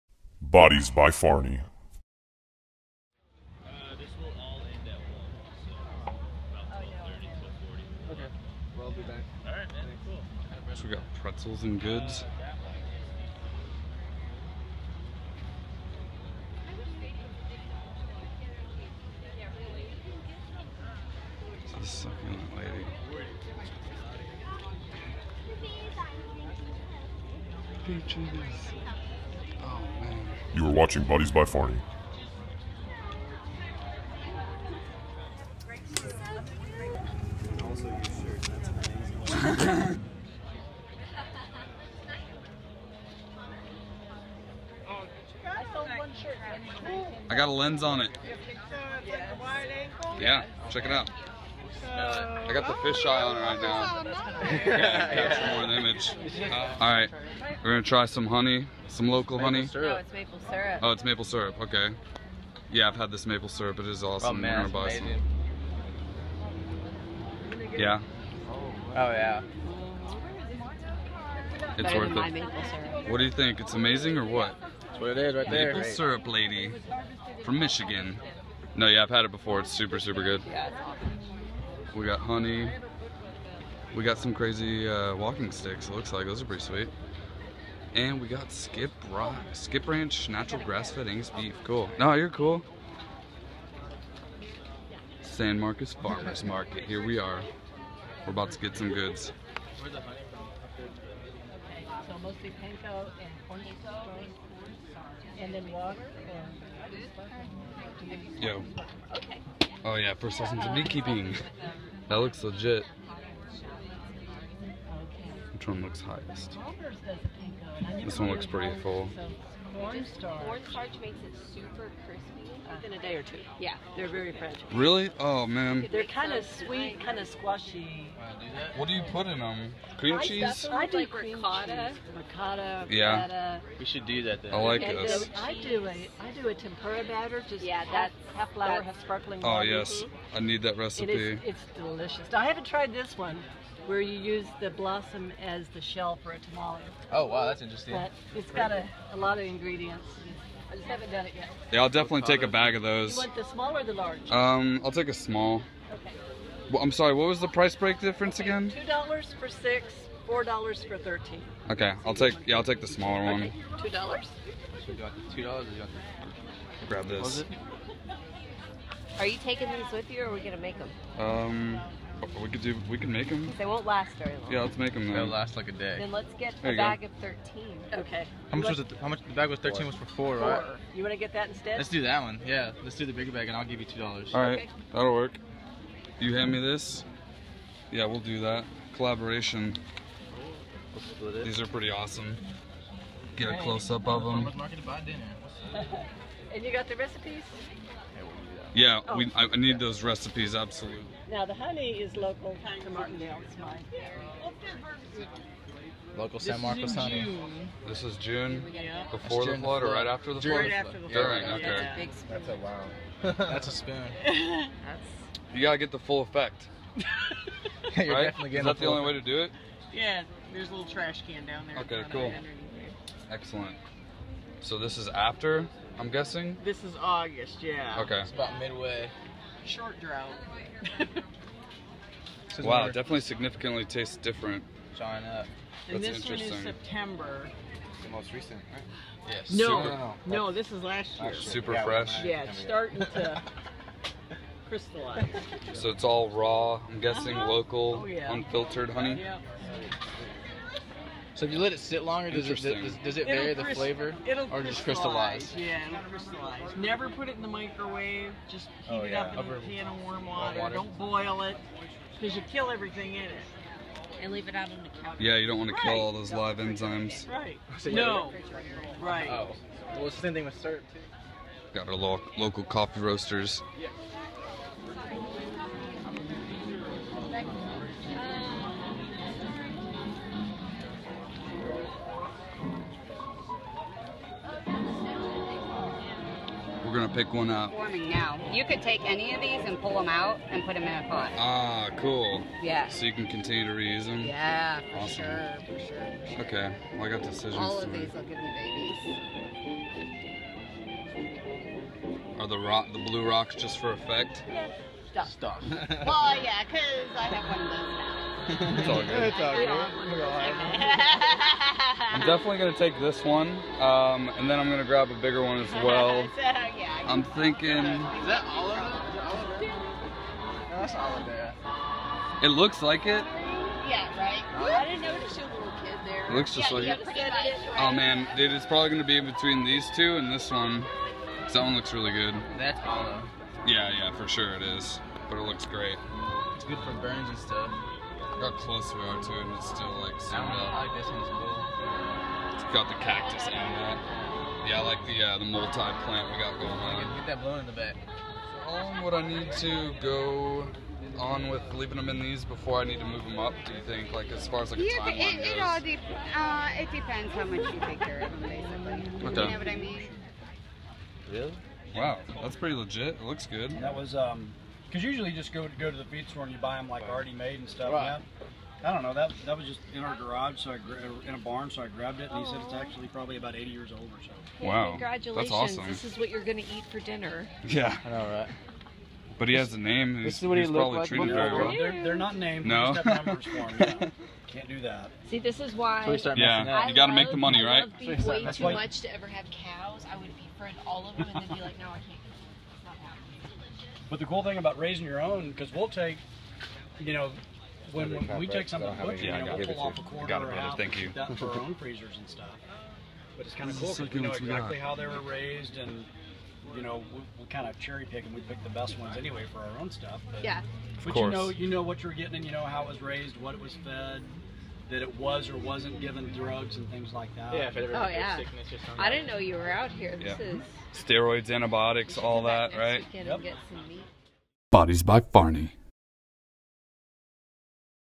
San Marcos Texas Farmers Market August 2015
I was in town for the weekend floating the Guadalupe with friends and decided I needed to get some local goods from the local San Marcos TX farmers market. During the visit I purchased Squash Blossums which are an edible flower you can stuff Ricotta cheese and other types of cheese in. Maple Syrup Lady's Michigan maple syrup. Local organic peaches from Caskey Orchards and some Succulents from Suzie. Since I was traveling I didn't want to get too much produce. Overall It was a good experience and nice to see some different vendors out.